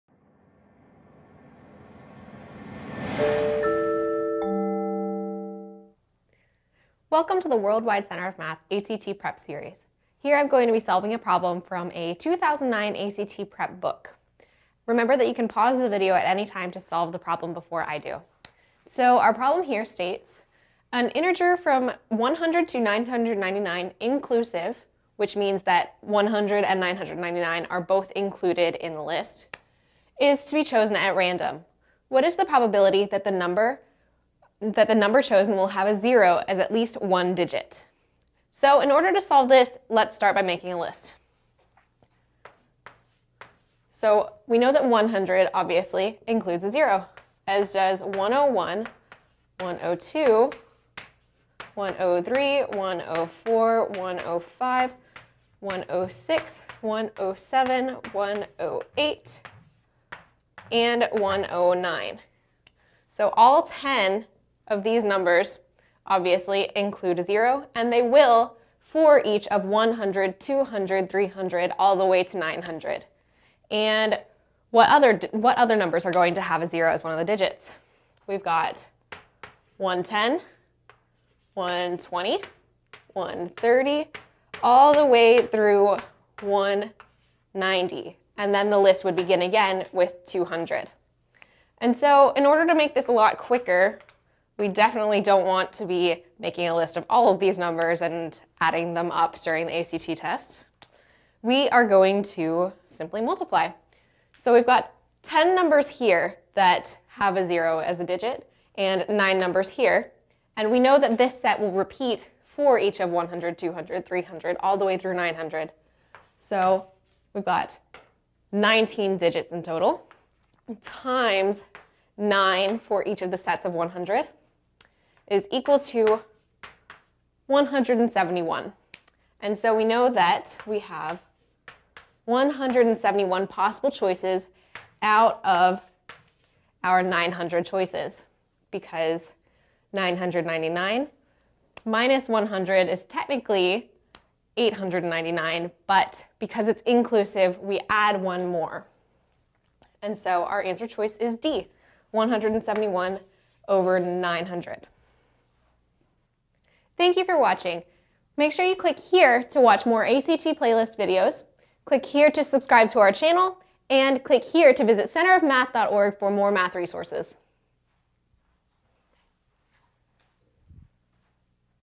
ACT Prep 10
Here math intern Tori solves a problem from a 2009 ACT test prep book.